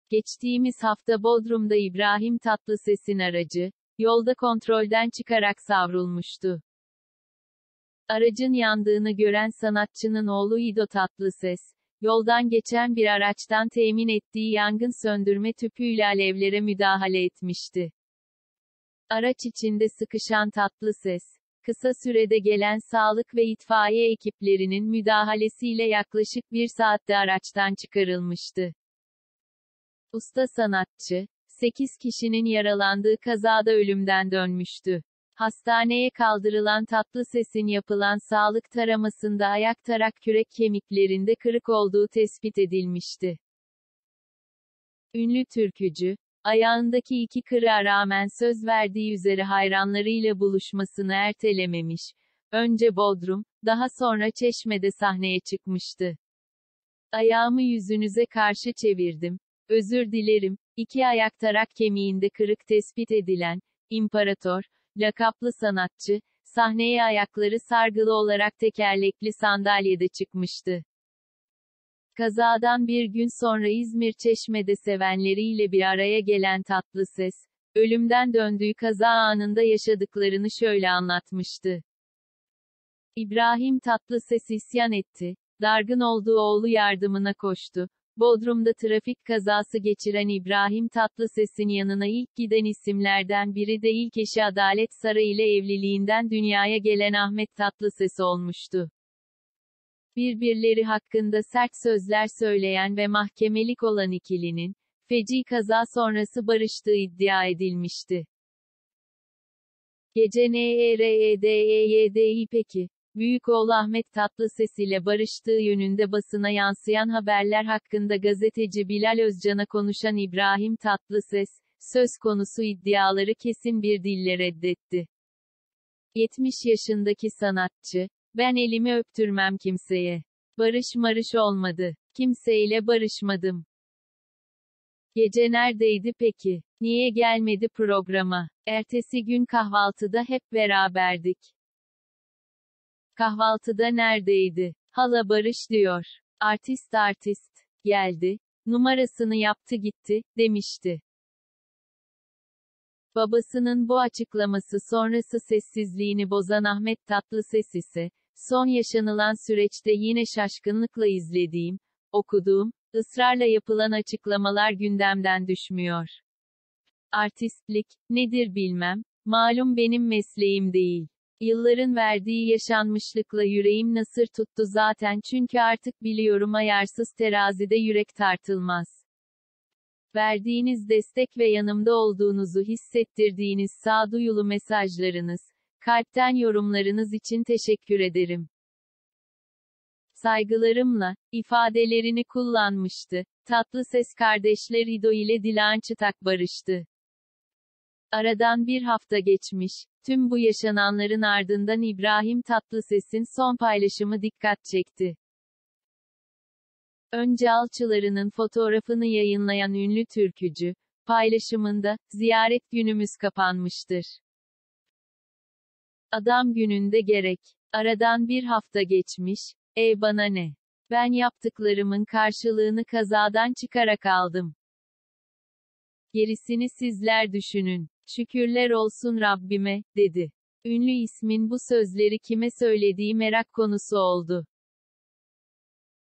🌏İbrahim Tatlıses’ten dostlarına ince sitem
Geçtiğimiz hafta Bodrum’da İbrahim Tatlıses’in aracı, yolda kontrolden çıkarak savrulmuştu. Aracın yandığını gören sanatçının oğlu İdo Tatlıses, yoldan geçen bir araçtan temin ettiği yangın söndürme tüpüyle alevlere müdahale etmişti. Araç içinde sıkışan Tatlıses, kısa sürede gelen sağlık ve itfaiye ekiplerinin müdahalesiyle yaklaşık bir saatte araçtan çıkarılmıştı.